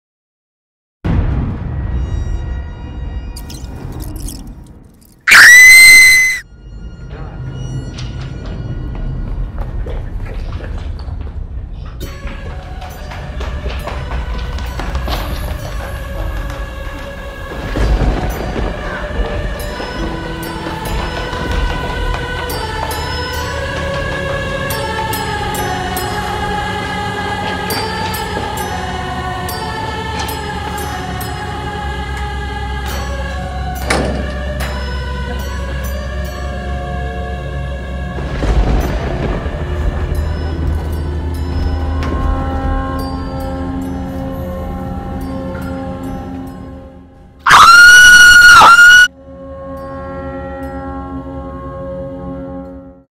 Silent Movie
AV Tech & Film project where we couldn't have dialogue and it could only be 45 seconds long, not counting the credits, so my group did a "horror film". The project was pretty fun, I looked like Cousin IT.